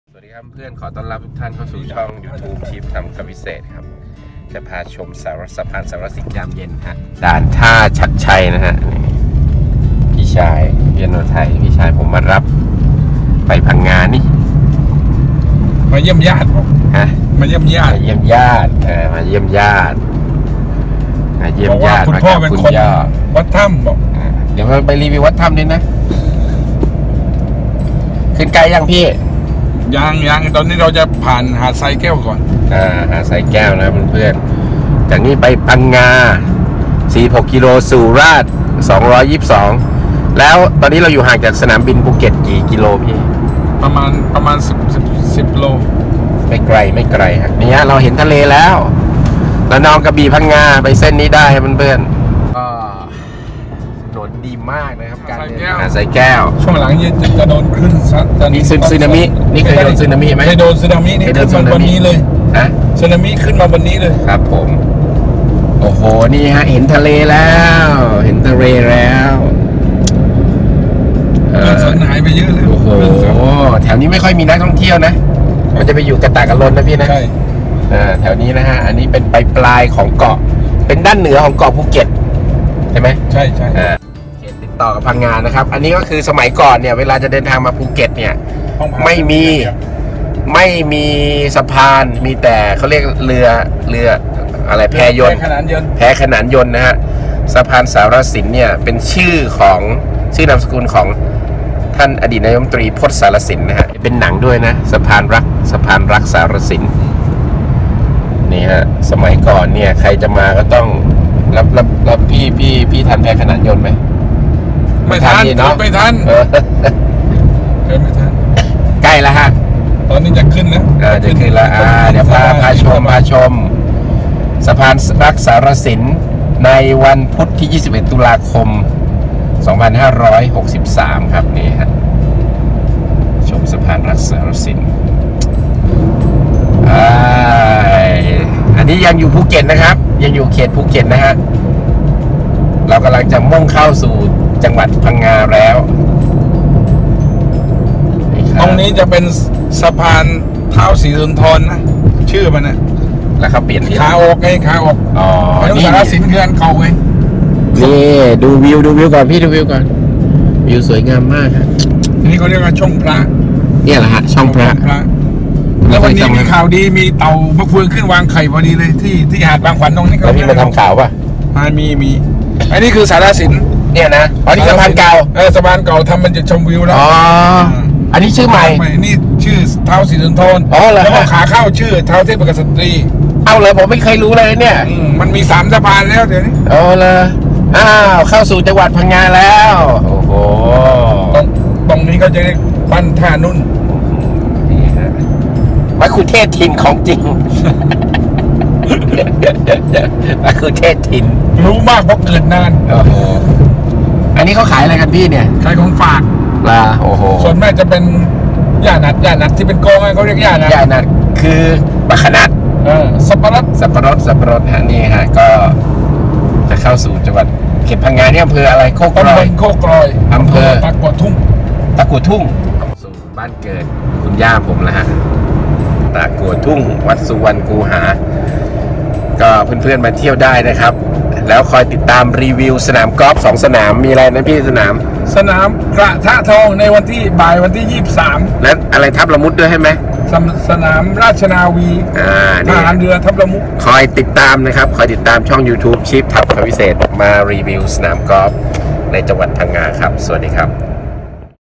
เที่ยวภูเก็ต สะพานสารสิน ชื่อมาจาก พจน์ สารสิน อดีตนายกรัฐมนตรี เชื่อมเกาะภูเก็ต กับ จ.พังงา
พุธ 21 ตุลาคม 2563